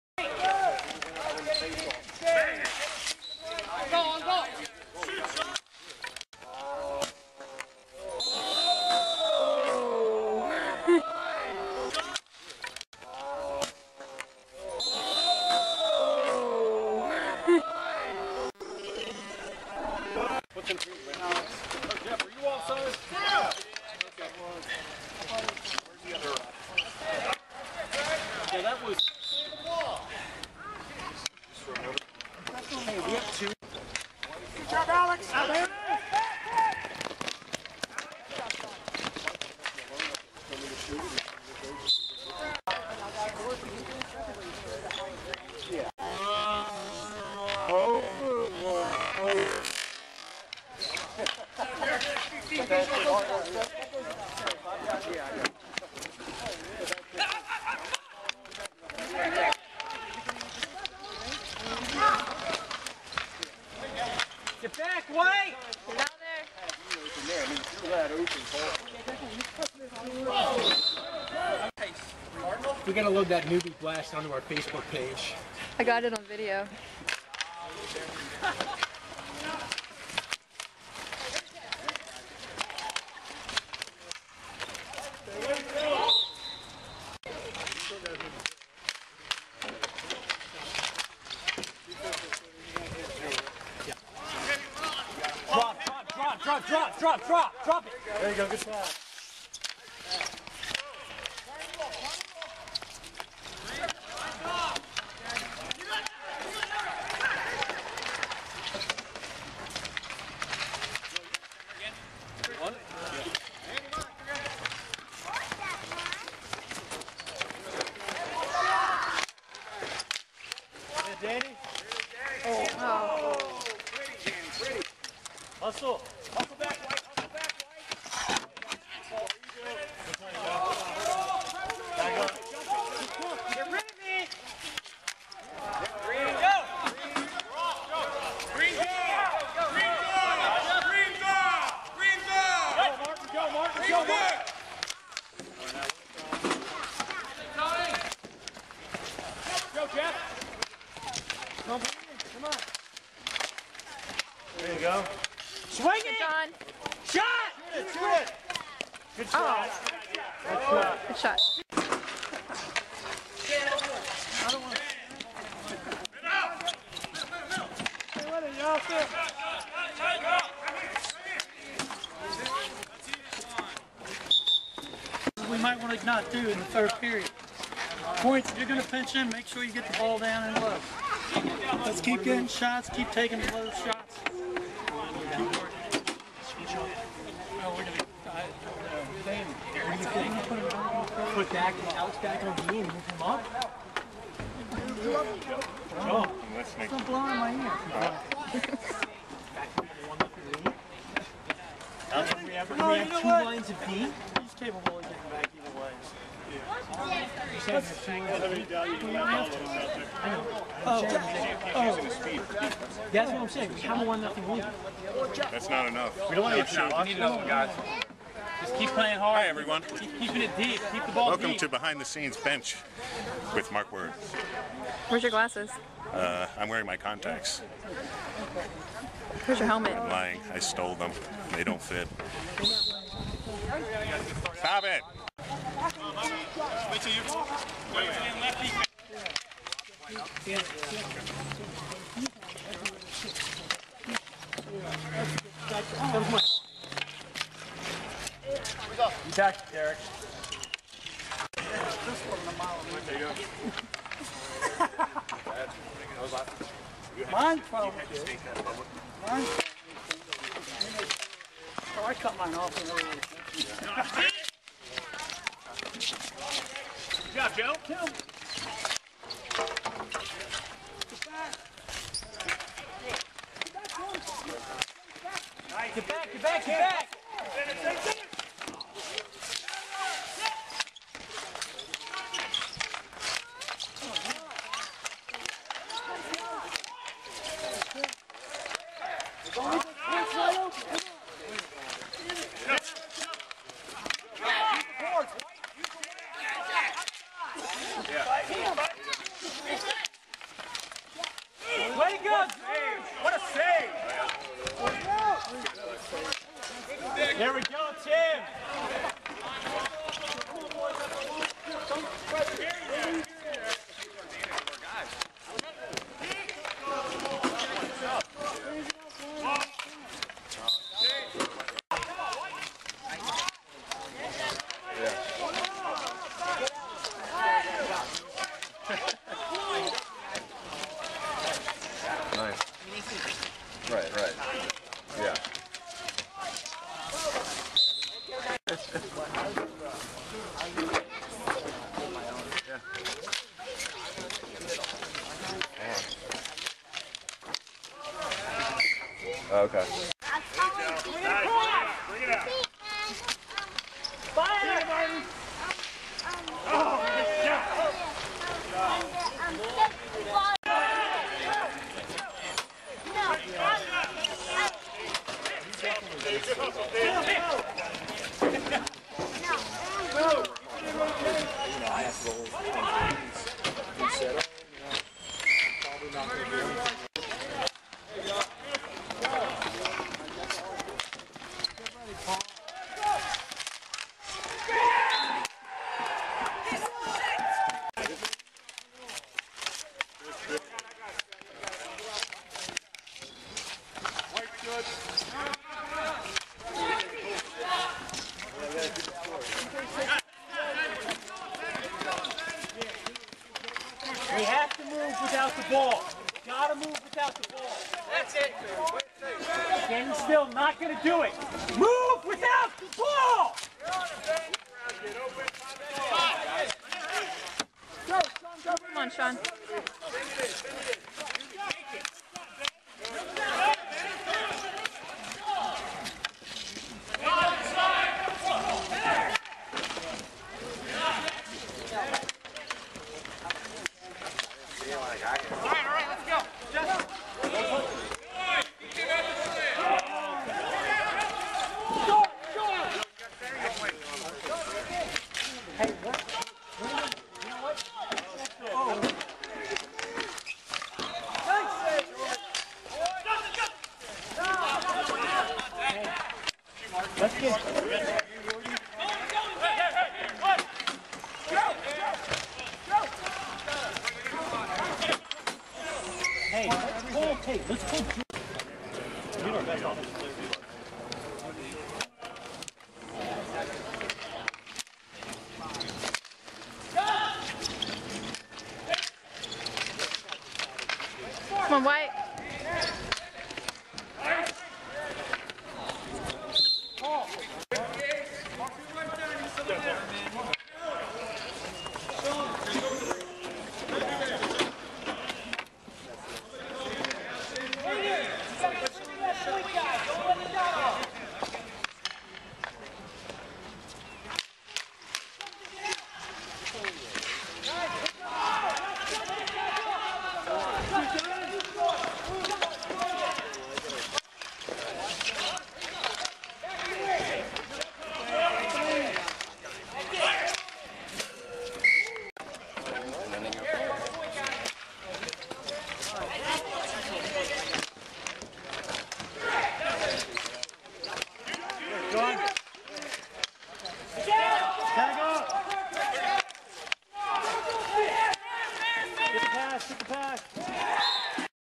street hockey part 2
more street hockey footage, see the broken window again